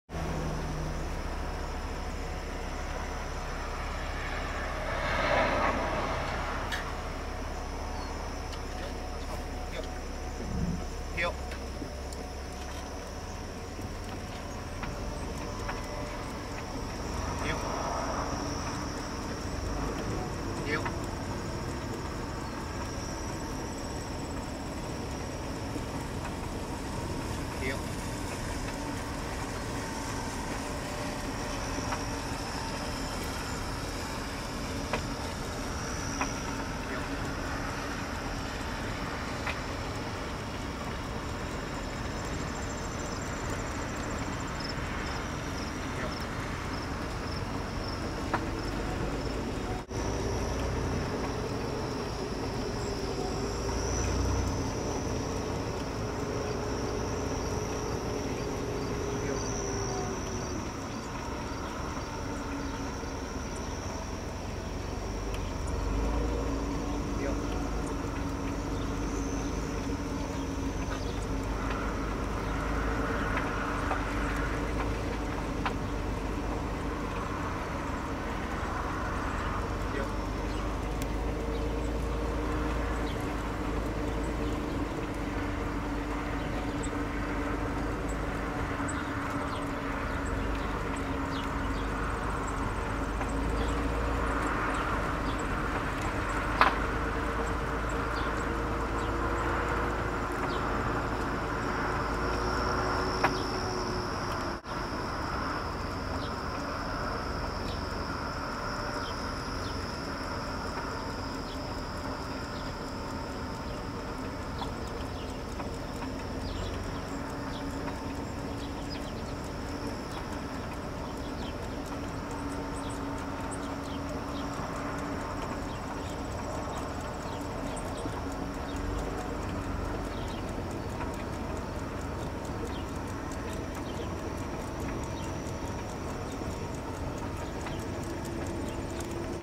Protection Dogs CCPD - Belgian Malinois Personal Protection Dog Cindy going for a Bike Ride
In this video, we demonstrate how to exercise your Protection Dog with a bike. To exercise your security dog with a bike is a great way to burn up that pend up energy.